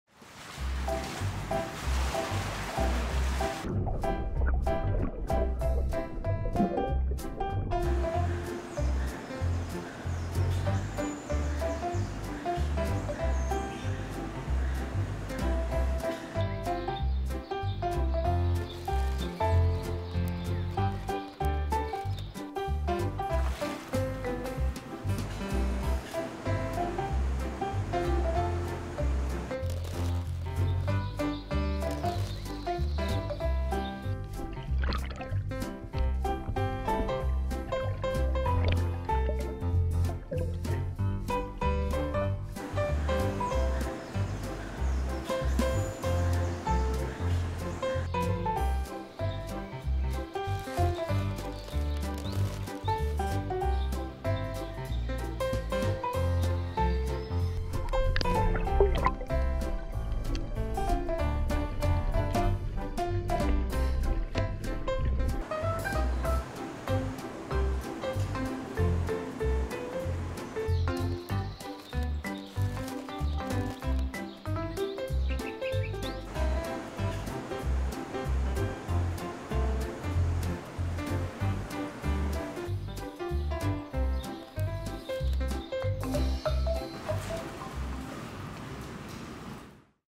Summer Ice Treats
Summer is still in full swing around here. Our animals got some tasty ice treats recently to cool off and enjoy!